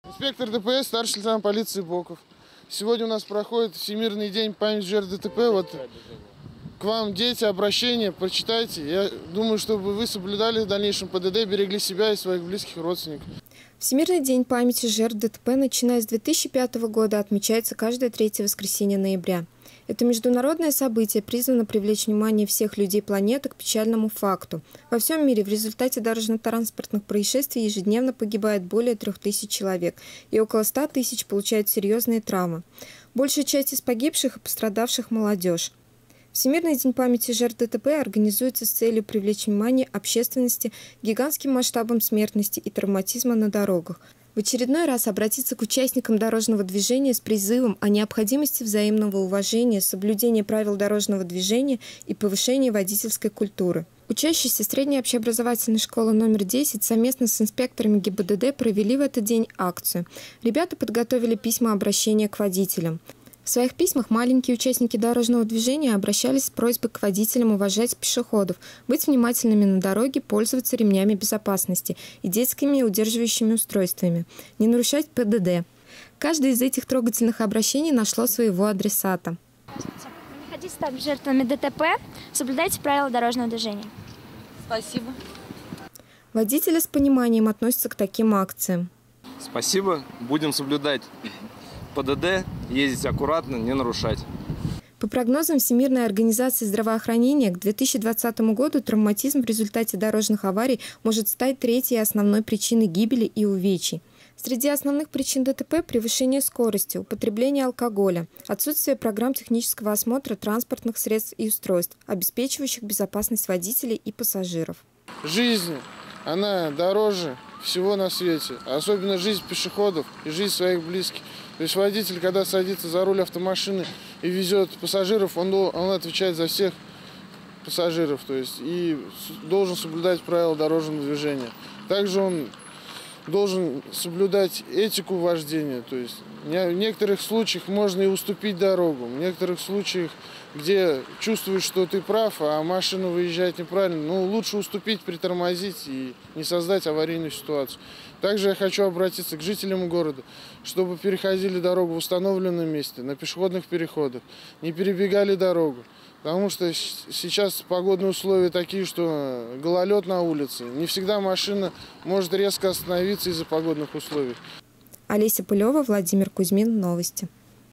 АРИС День памяти жертв ДТП
Груда искореженного металла мало похожая на автомобиль. Такую экспозицию выставили сотрудники ГИБДД возле Вечного огня. Цель акции – почтить память погибших в ДТП и напомнить автолюбителям о необходимости соблюдения правил дорожного движения.